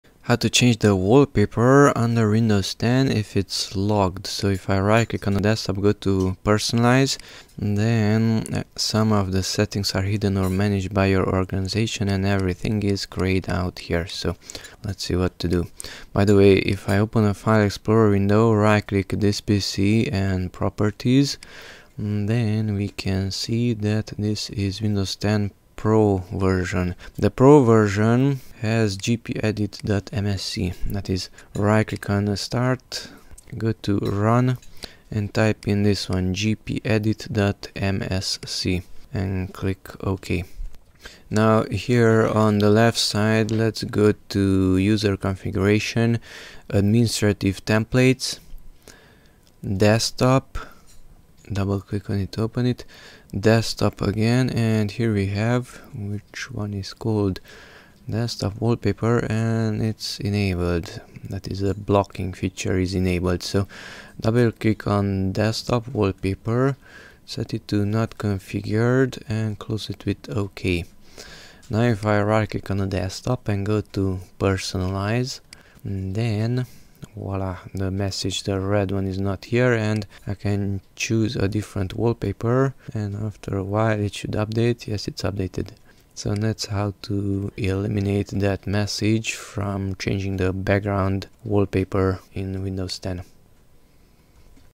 How to replace Wallpaper? (Some of these settings are hidden or managed by your organization)
gpedit.msc

- User configuration
- Administrative templates
- Desktop
- Desktop
- Desktop Wallpaper (Not configured)